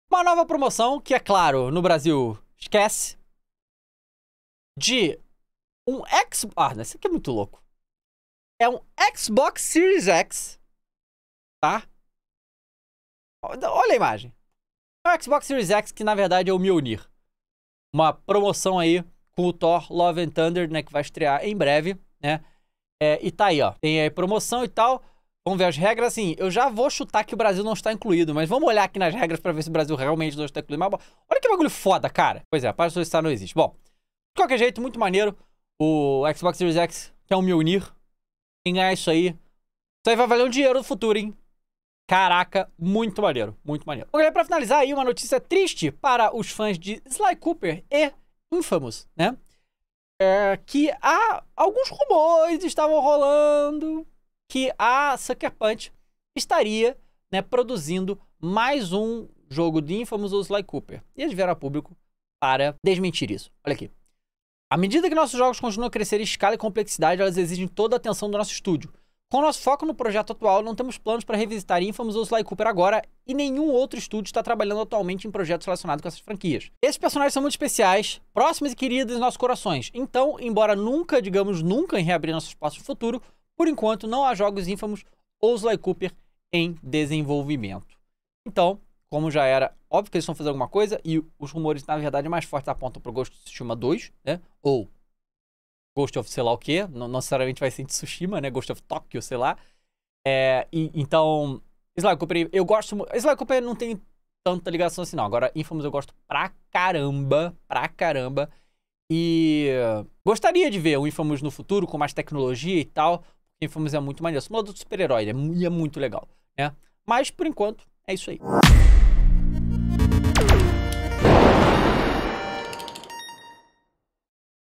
DIA TRISTE para ser BRASILEIRO
Fala galera! No vídeo de hoje teremos um corte comentando sobre uma promoção do martelo do Thor.